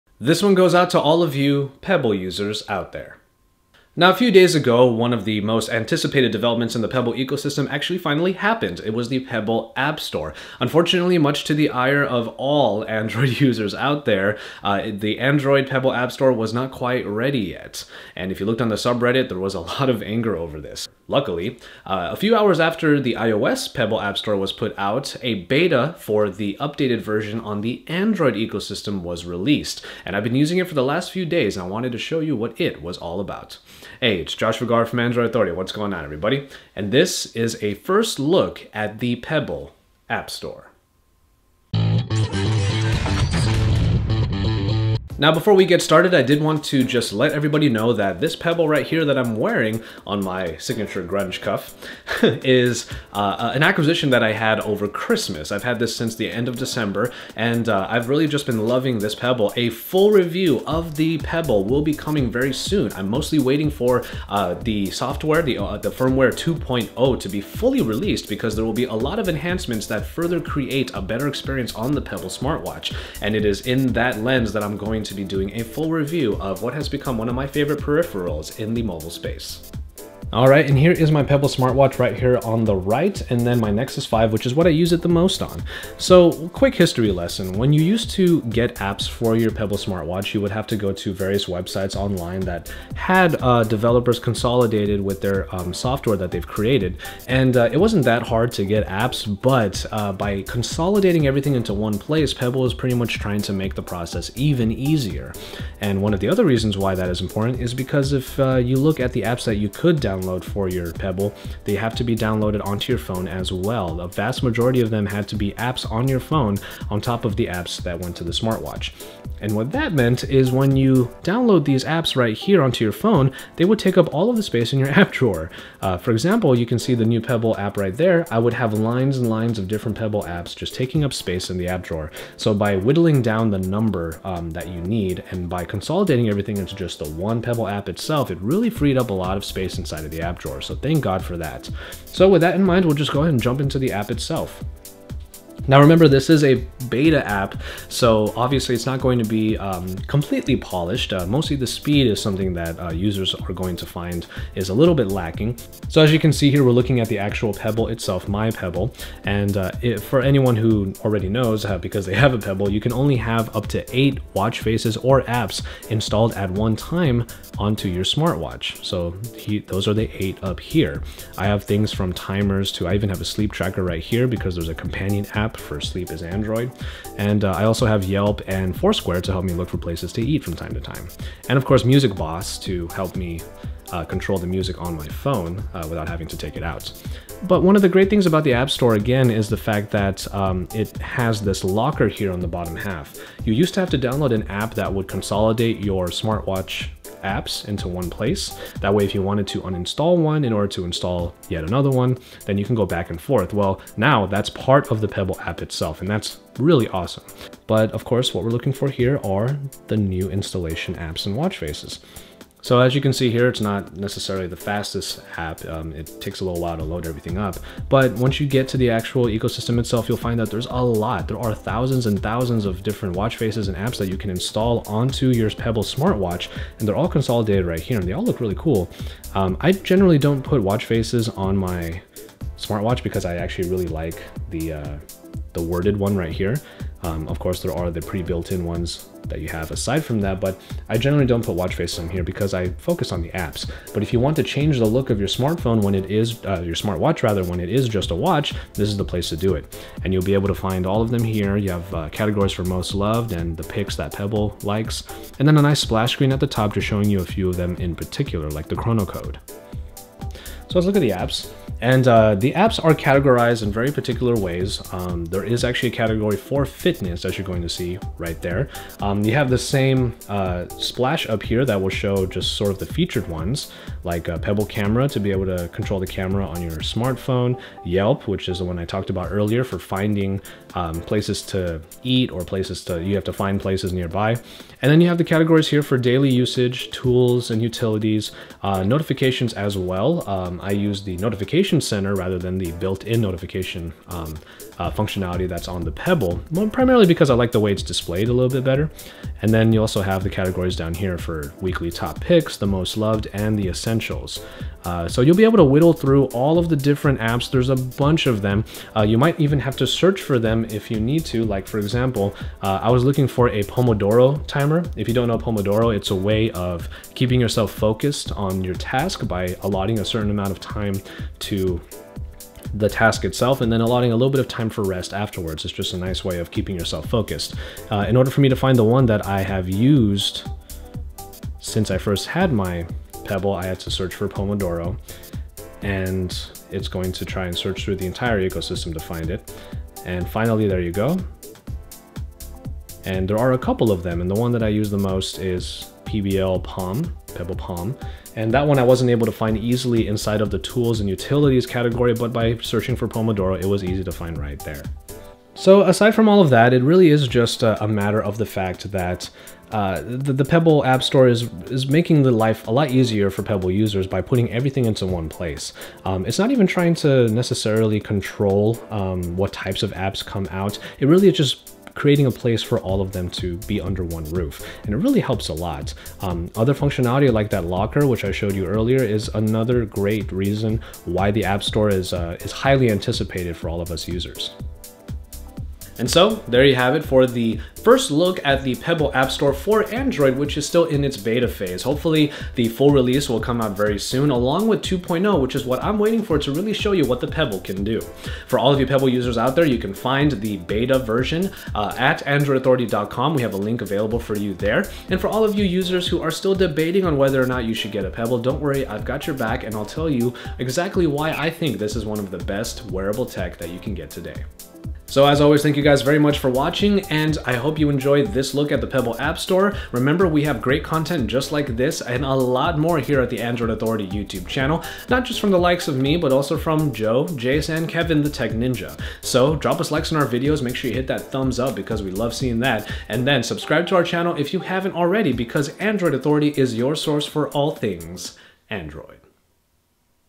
Pebble App Store for Android - First Look!
The Pebble is a wonderful piece of wearable tech and Josh (along with every other Android Pebble user) has been anticipating the new firmware and app store release. We might not have gotten a full release this week, but the beta definitely shows what we could look forward to in Pebble world!